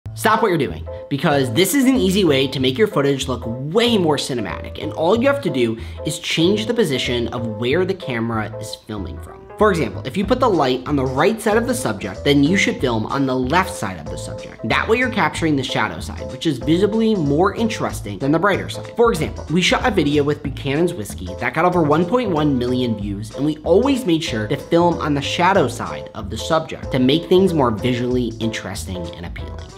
Cinematic Video Tip: Film on the Shadow Side
Stop what you’re doing because this is an easy way to level up all your shots by making them look more cinematic. And it all it requires you to do is change the position of where the camera is filming from.

For example, if I put your light here, you should put your camera here, about the subject. That way you’re capturing the shadow side, which is visually more interesting than the bright side.

For example, we shot a video with Buchanan’s Whiskey that got over 1.1 million views, and we always made sure to film on the shadow side of the subject.